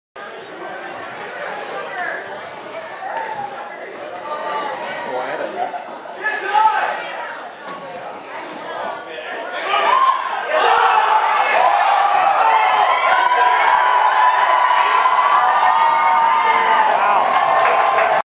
MSHS vs Interlake HS 2010 - #11 Michael Blackmon - GOAL
Mount Si High School Boys Varsity Soccer. Snoquamie, WA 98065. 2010 Season. Member KingCo 3A/2A Conference